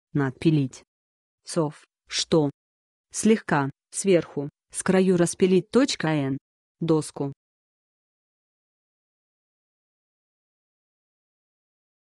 НАДПИЛИТЬ - что это такое? значение и описание
Хочешь знать что значит НАДПИЛИТЬ? тогда слушай до конца.